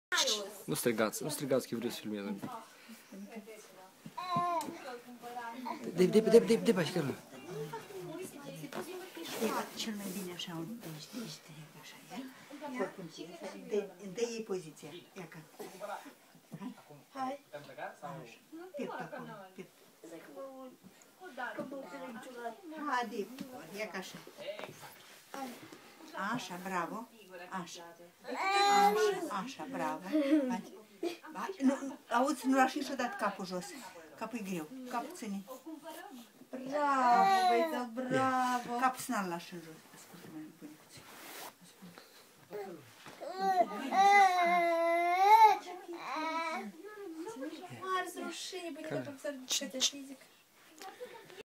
Matei Marsejnîi la prima tentativă de a merge în patru.În a
Matei Marsejnîi la prima tentativă de a merge în patru.În ajun de 2013.